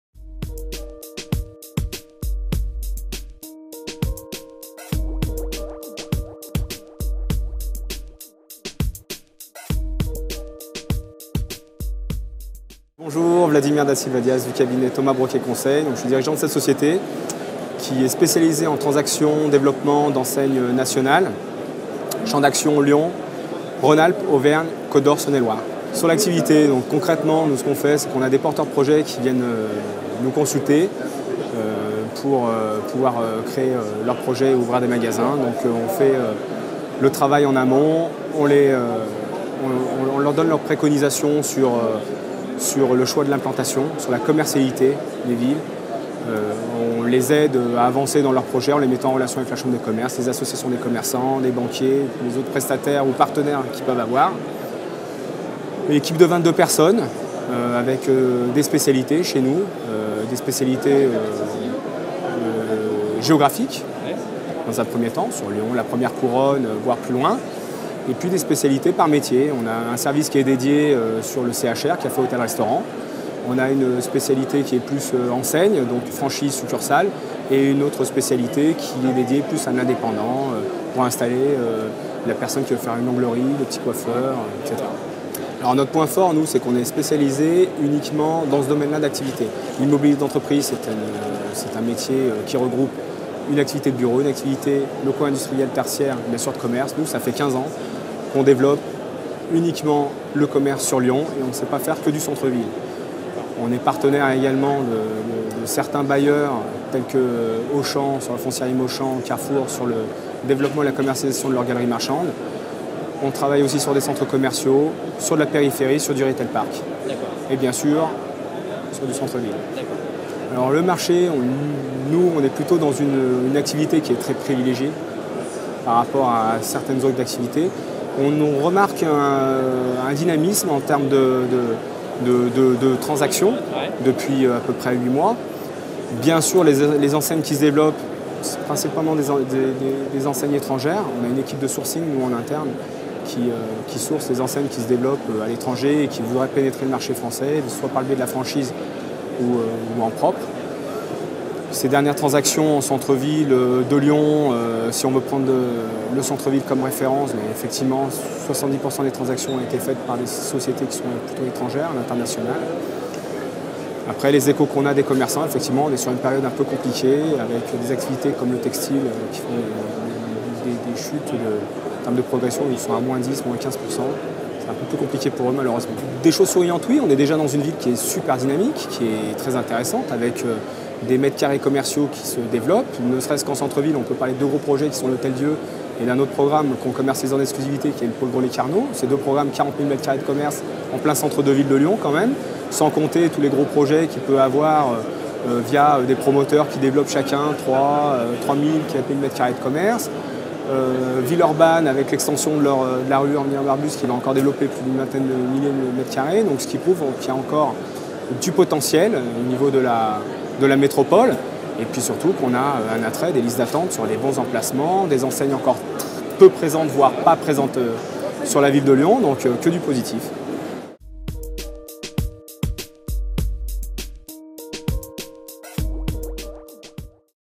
Vladimir Da Silvia Dias - Thomas Broquet Conseils
A l'occasion du Forum Franchise qui s'est déroulé mercredi 19 octobre 2016, Vladimir DA SILVIA DIAS présente Thomas Broquet Conseils sur l'atelier Flash Franchise